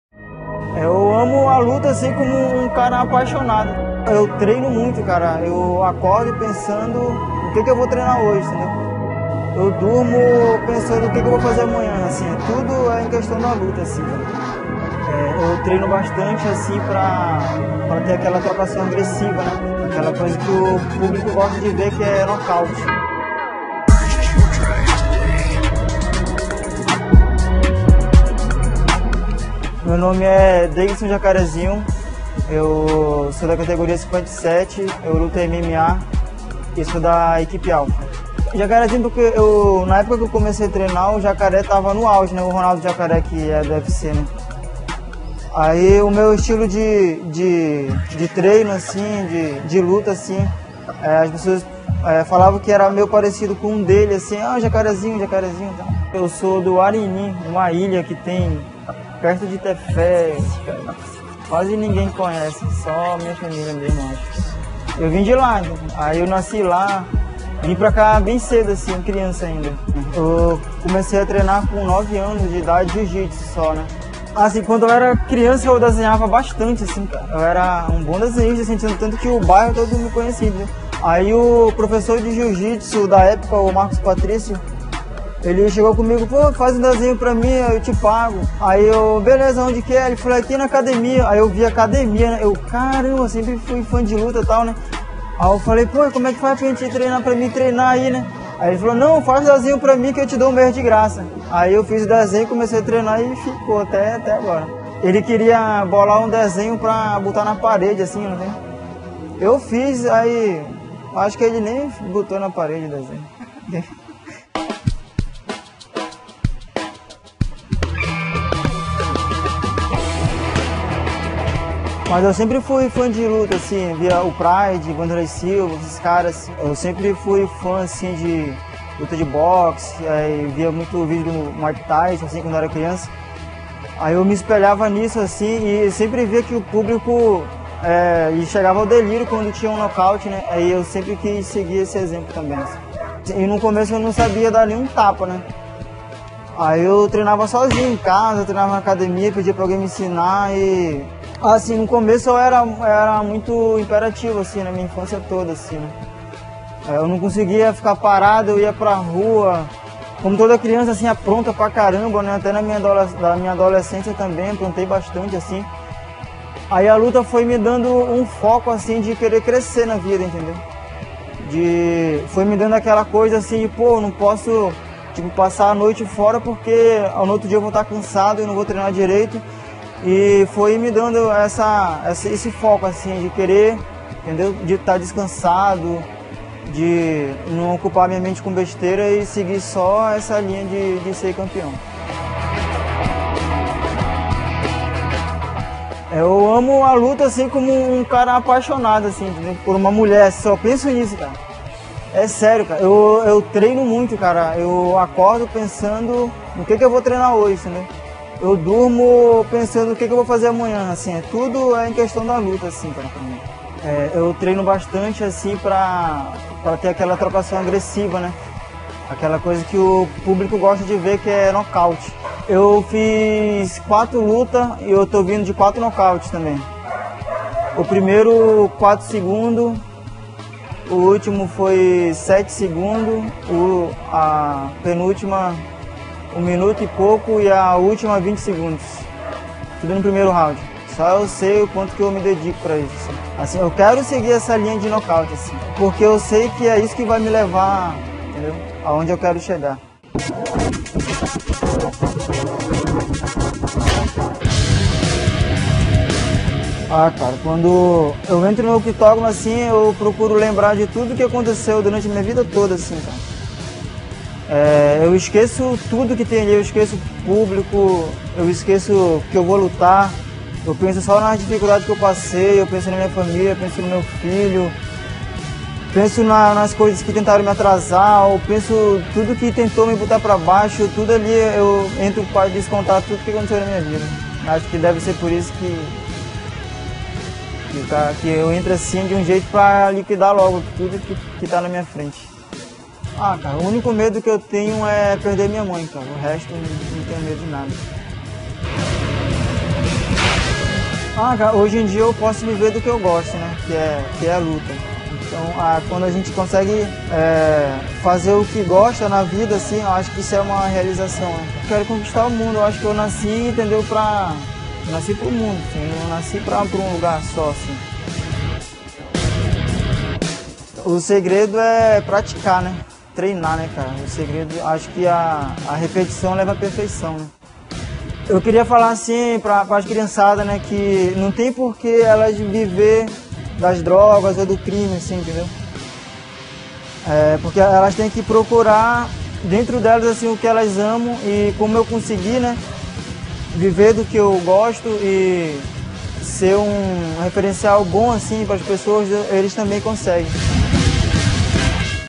Esporte Amazônia | Atleta amazonense vem se destacando no MMA
Jacarezinho atleta de MMA vem se destacando no MMA